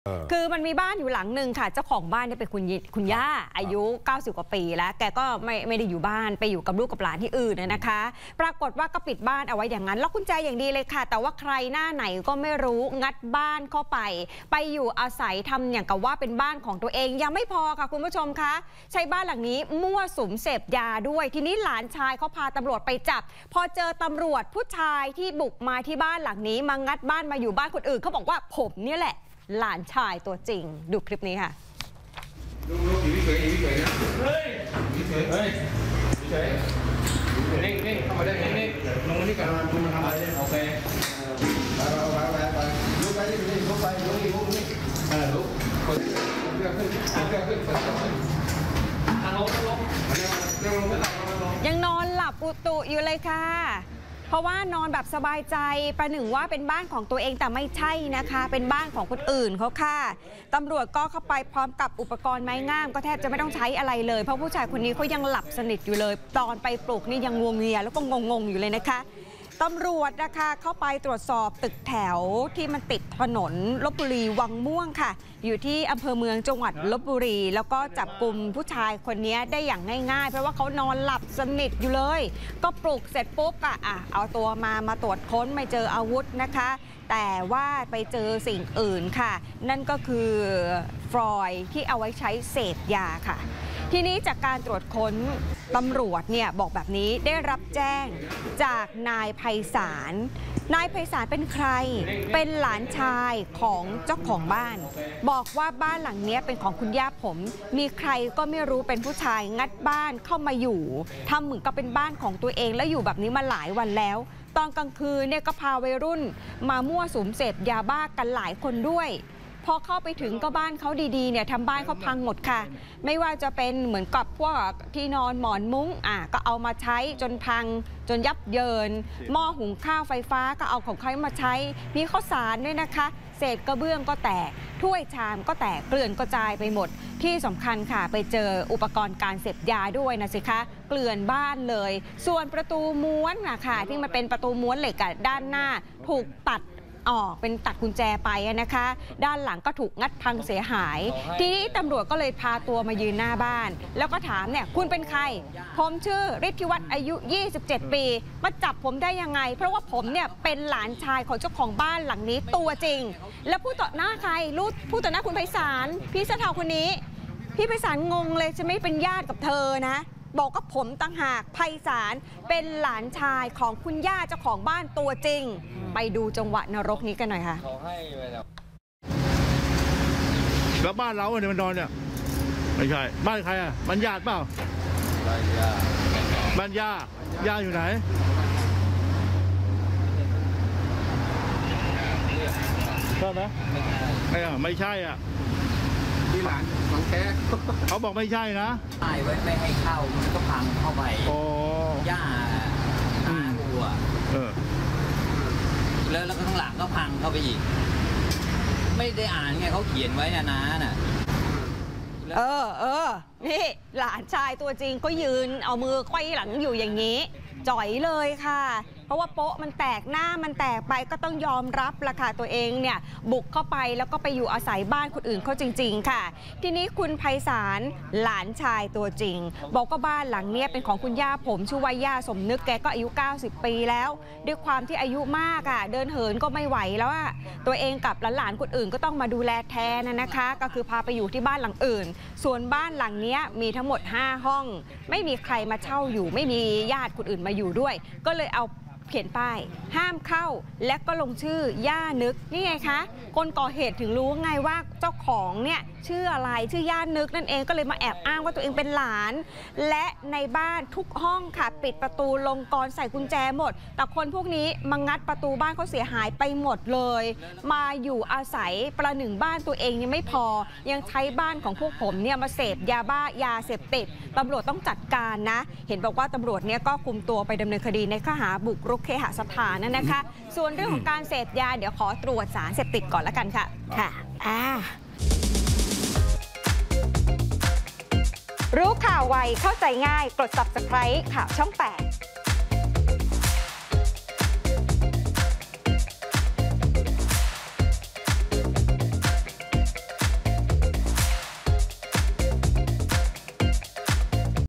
จับกุมหนุ่มแก๊งยาบ้า ยึดบ้านคนอื่นตีเนียนอ้างเป็นหลานเจ้าของบ้าน | ข่าวช่อง8
หลานเจ้าของบ้าน แจ้งตำรวจจับแก๊งขี้ยา งัดบ้านย่า เข้าไปนอนมั่วสุมกว่า 10 คืน ตำรวจไปเจอ อ้างว่า "ผมนี่แหละ หลานเจ้าของบ้าน" ใครเป็นตัวจริงตัวปลอม?